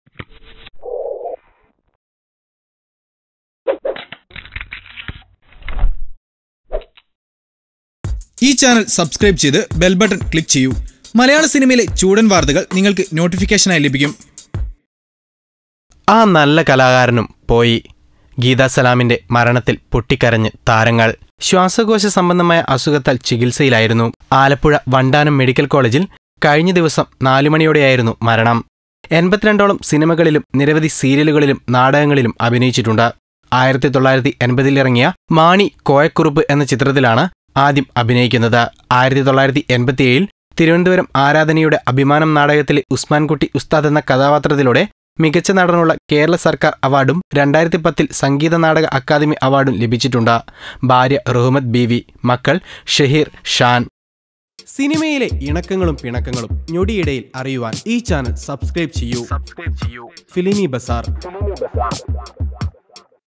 ആ നടനും പോയി ഗീത സലാമിൻ്റെ മരണത്തിൽ പൊട്ടിക്കരഞ്ഞ് താരങ്ങൾ | Actor Geetha Salam
Watch ആ നല്ല കലാകാരനും പോയി നടൻ ഗീത സലാമിൻ്റെ മരണത്തിൽ പൊട്ടിക്കരഞ്ഞ് താരങ്ങൾ Actor Geetha Salam